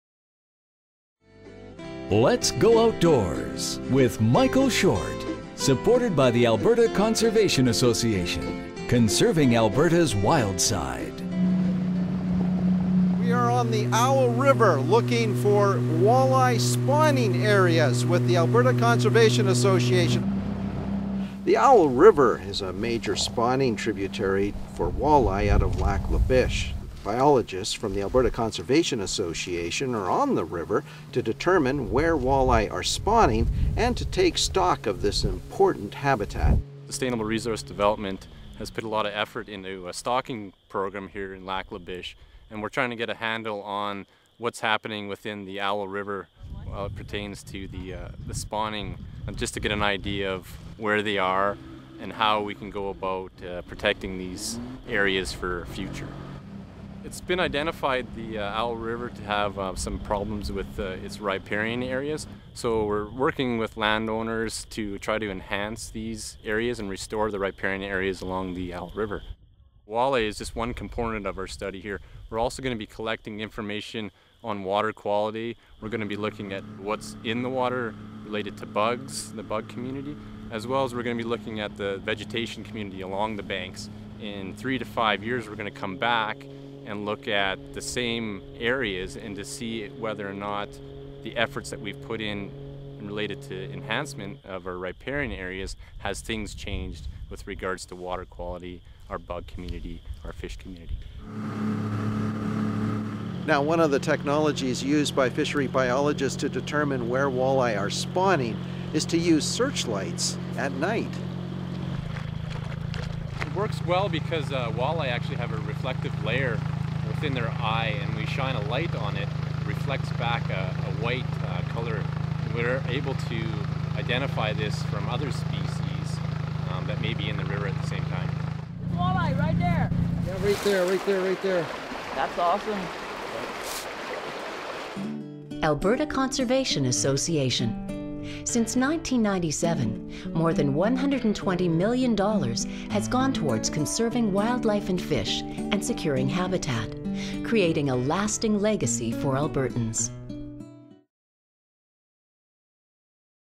ACA Owl River Walleye Study
See what Alberta Conservation Association biologists get up to at 3 a.m.! This spring, we did a walleye spawning assessment in the early morning hours using boats and spotlights to help us see the fish. It was the first step in this multi-year project to restore riparian vegetation and hopefully take some pressure off Lac La Biche's walleye stock.

The Owl River is a primary spawning river for Lac La Biche walleye, but unfortunately the river also needs some riparian restoration. Parts of the river's walleye spawning area run through land used for livestock grazing. Long-term grazing in the area has reduced riparian vegetation and increased nutrient loading in the river. Our work to restore the riparian vegetation should give the spawning fish better habitat and also reduce algae blooms caused by the river's current nutrient load to help fry migrate out to the lake more successfully. Our biologists plan to do this by working with landowners to fence off the river so that cattle can't reach it.

Video provided by Michael Short  from Let's Go Outdoors.